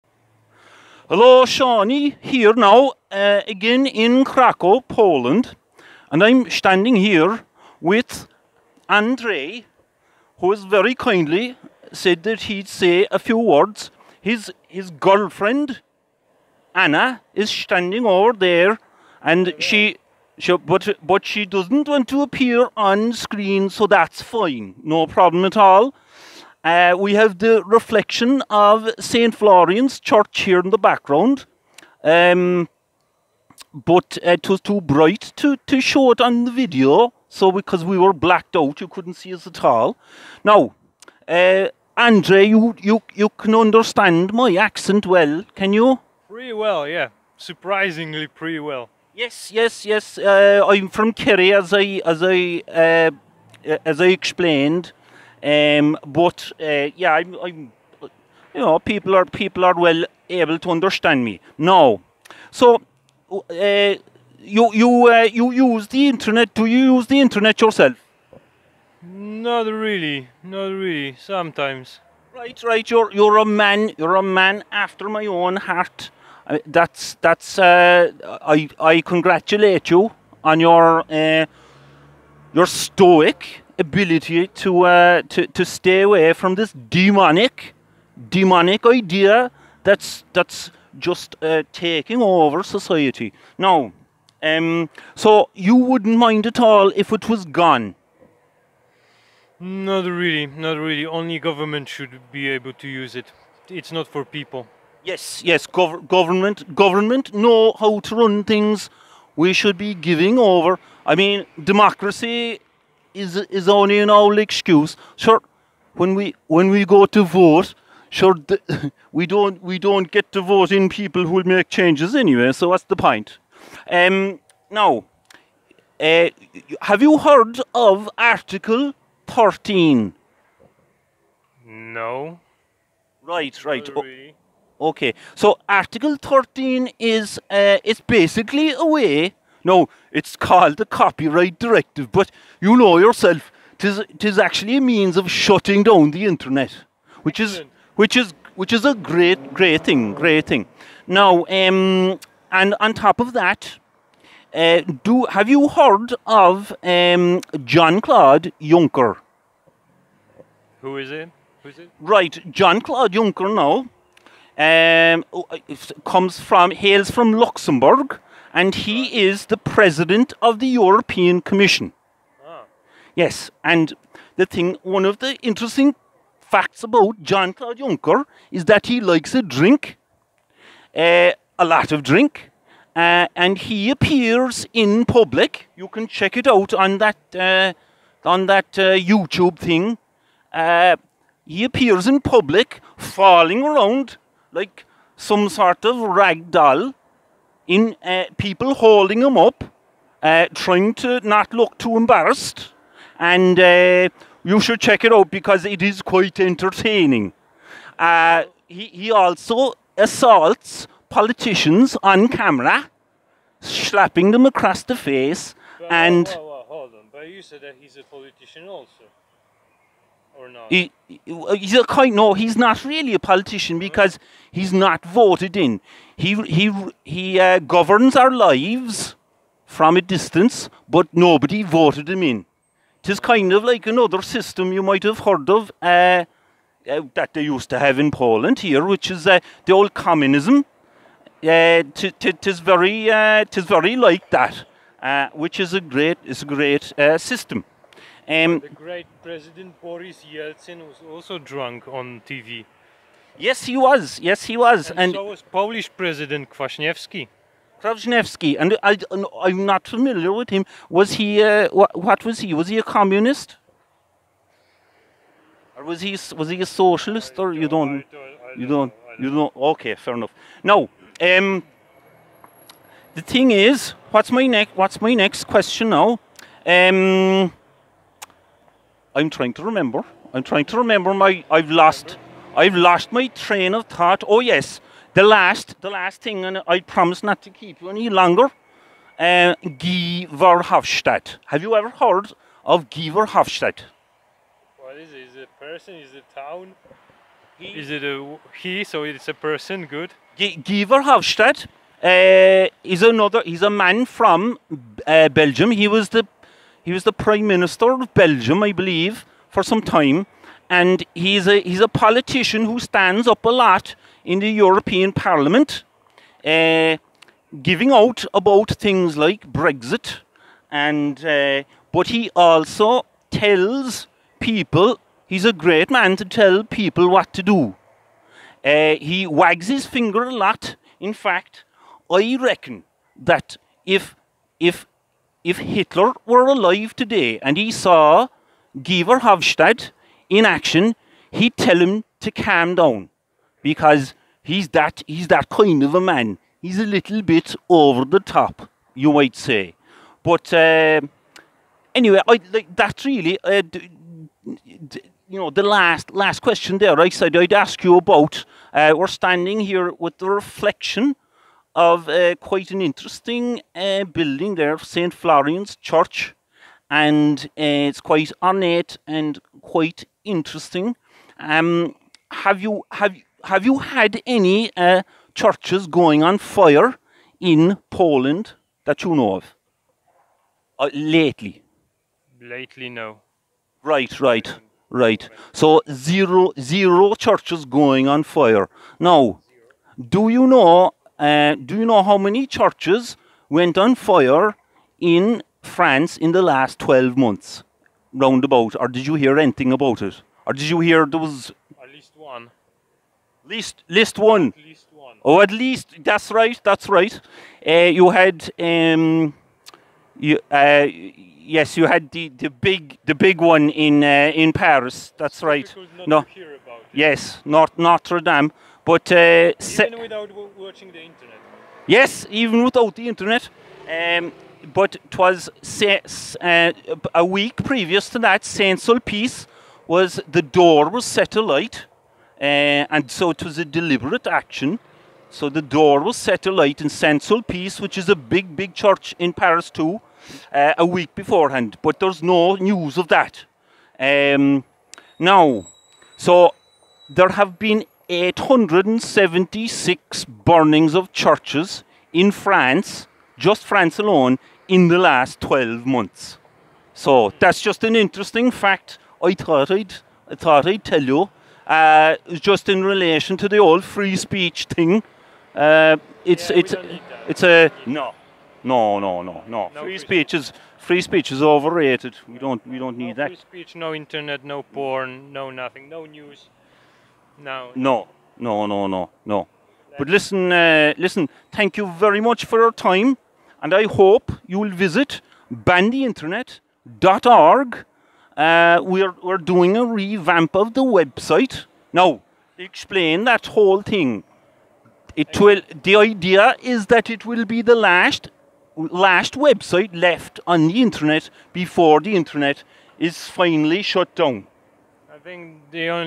Banning the Internet: What does Poland think?
Andrej (Hope I got the spelling right) is with Seánie on wanting to ban the internet. Fair play to him. (Anna his girlfriend is lending her support off-camera.)

This conversation looks at a few common subjects about the internet but also asks a few questions about churches. More on that later. Thanks for being such good fun.